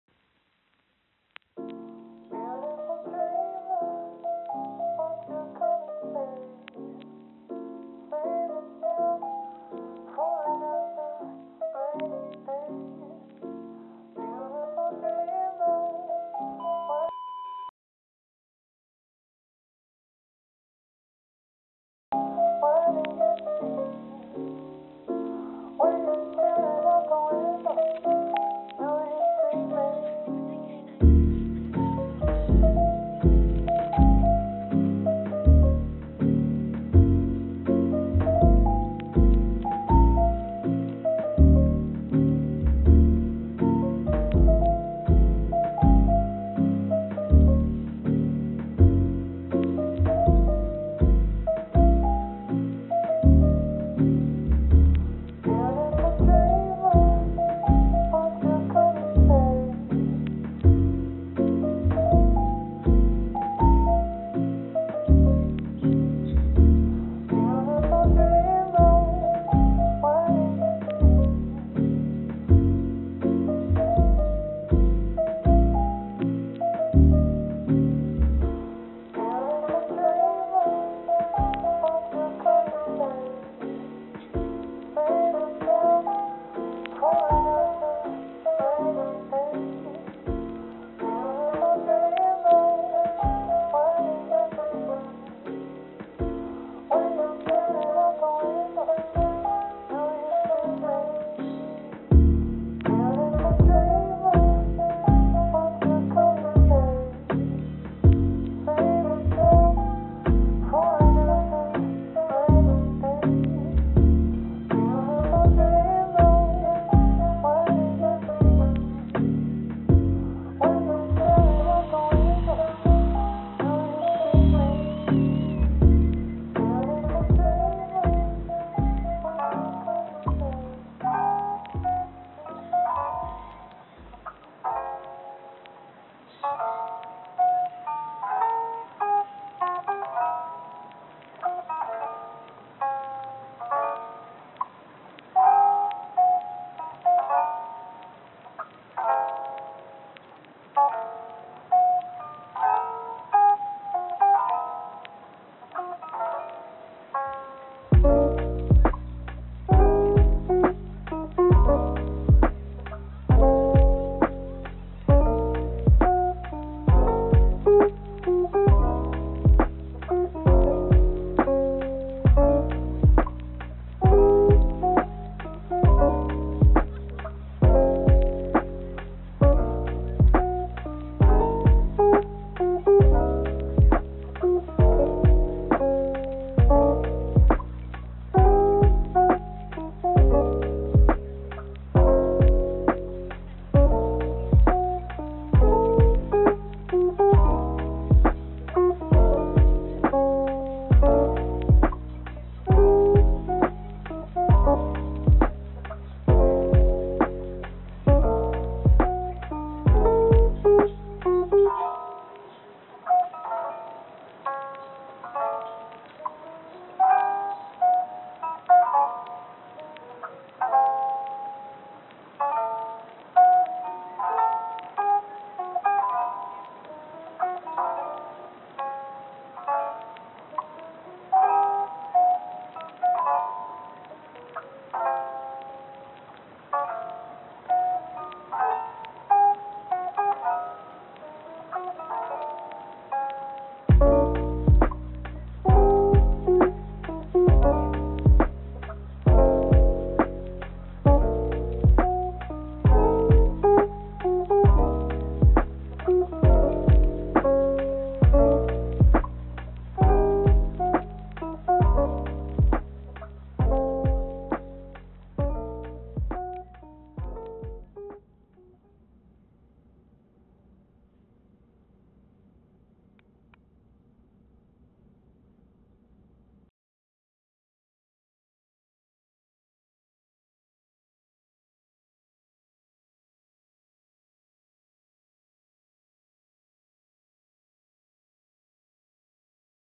cologne vlog, HOZIER concert
Literally waited for this moment for so long. One of the best concerts I've ever been too. Until next time.